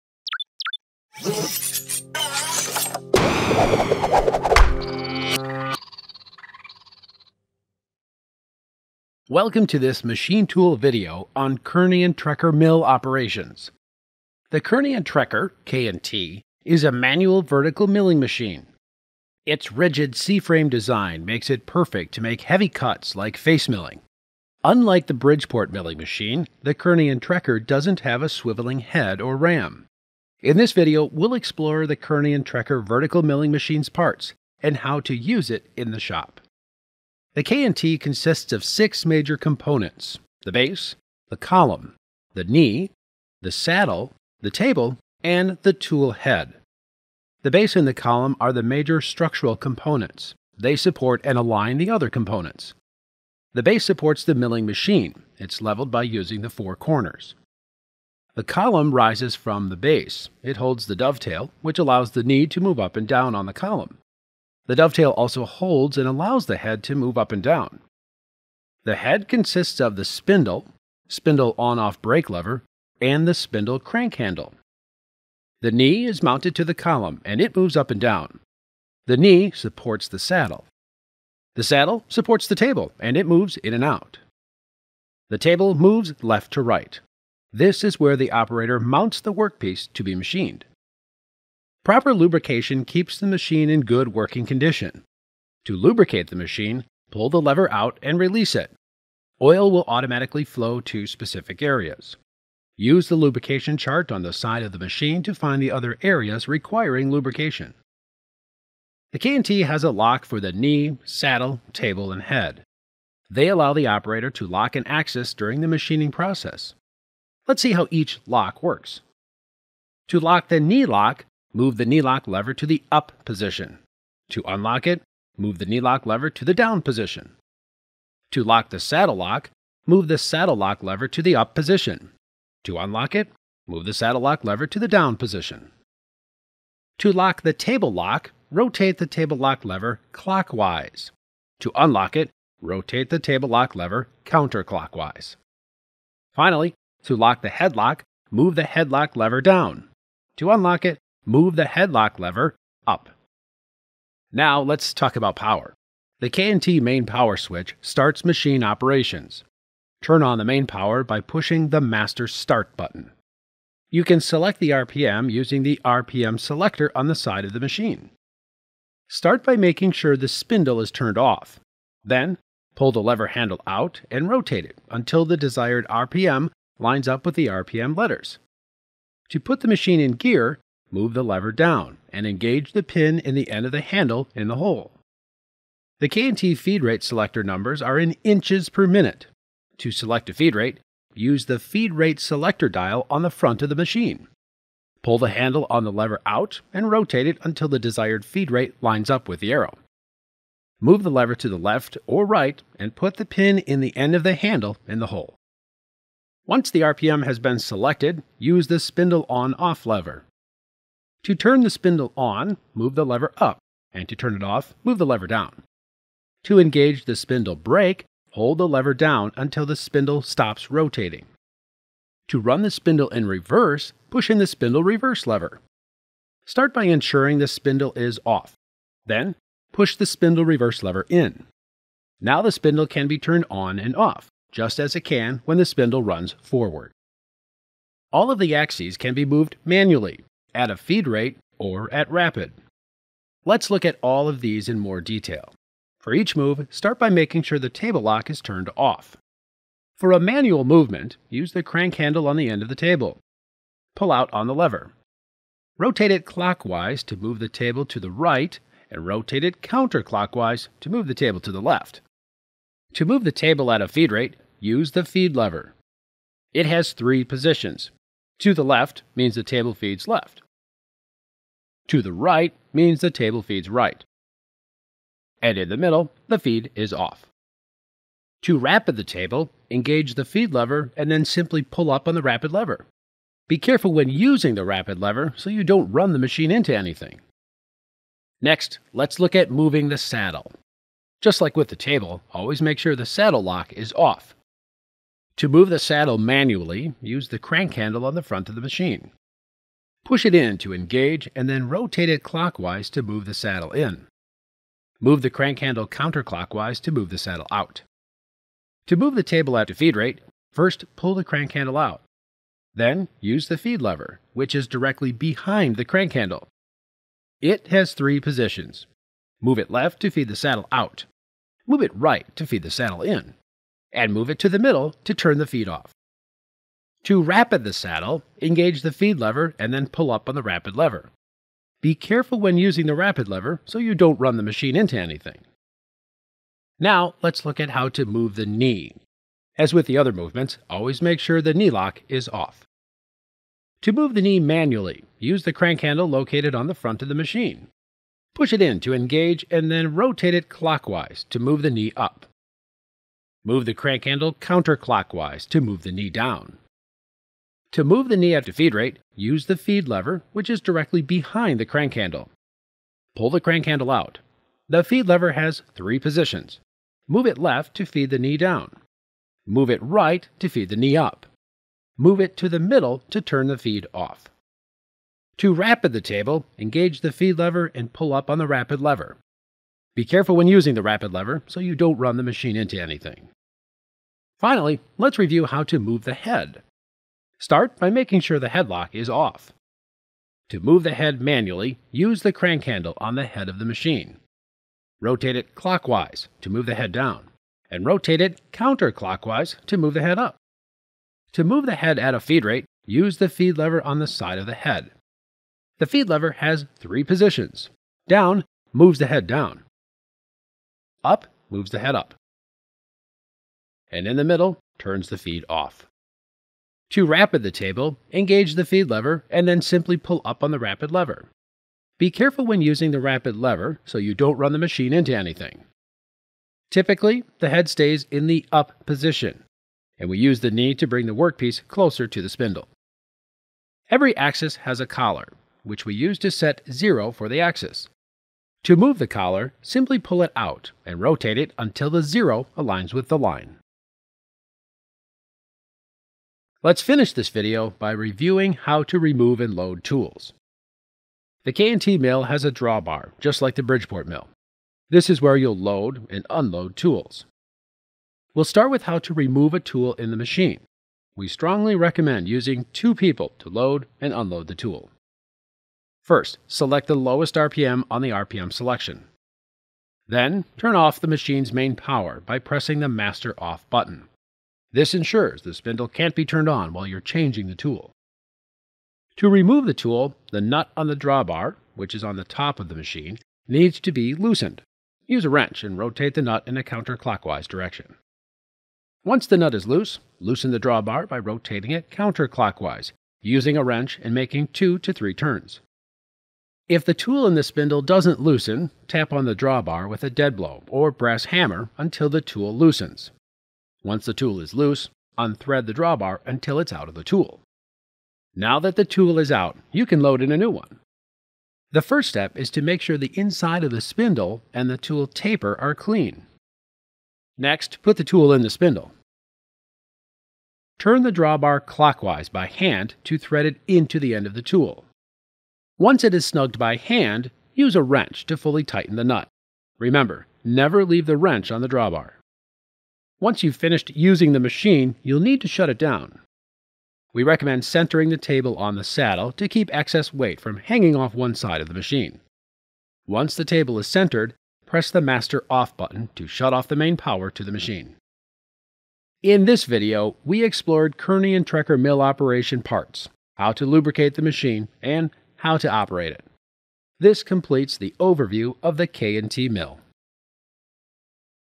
Kearney & Trecker Mill Operations
This video will walk you through operation of the Kearney & Trecker Milling Machine Operations.

Credits:
    - Content Author: Erich Krowas, FVTC Machine Tool Instructor
    - Video Developer: Chad Blohowiak